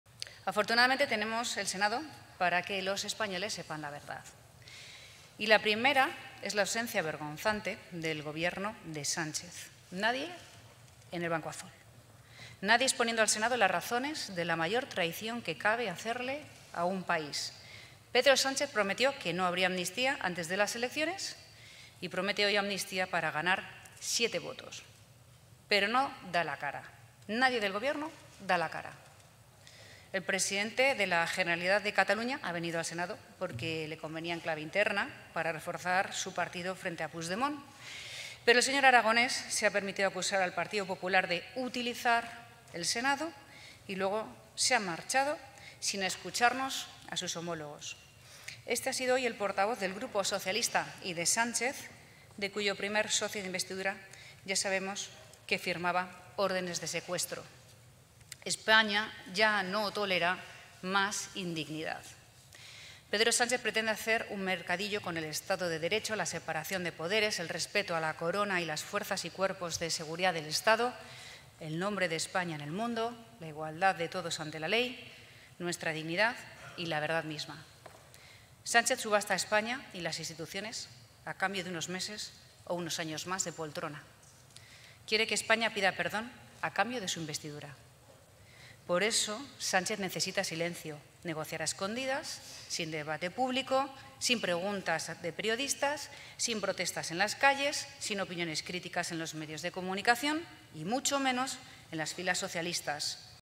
Ayuso carga contra Sánchez por querer hacer "un mercadillo con el Estado de Derecho"
La presidenta de la Comunidad y del PP de Madrid, Isabel Díaz Ayuso, ha cargado en el Senado contra el presidente del Gobierno en funciones, Pedro Sánchez, por pretender hacer "un mercadillo con el Estado de Derecho" y ha reiterado que considera la amnistía "la mayor traición que cabe hacerle a un país". "Sánchez subasta España y las instituciones a cambio de unos meses o unos años más de poltrona", ha sostenido.

(Fuente: Senado)